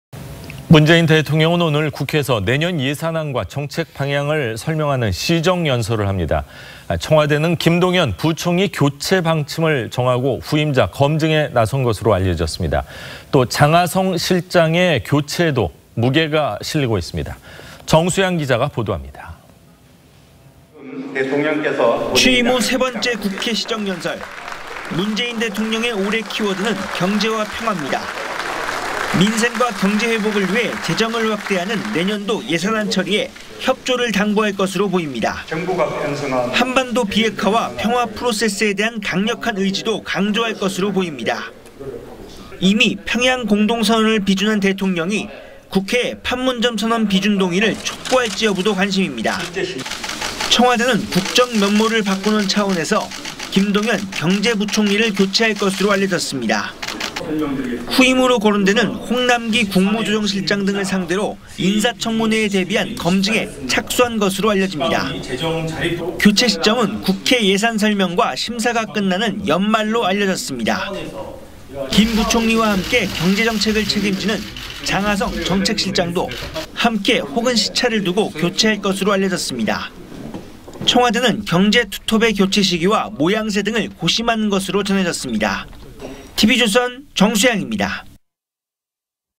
文대통령, 오늘 국회 시정연설…'판문점선언' 비준 거론 주목
문재인 대통령은 오늘 국회에서 내년 예산안과 정책 방향을 설명하는 시정연설을 합니다. 청와대는 김동연 부총리 교체 방침을 정하고 후임자 검증에 나선 것으로 알려졌습니다. 장하성 실장도 교체될 것으로 보입니다.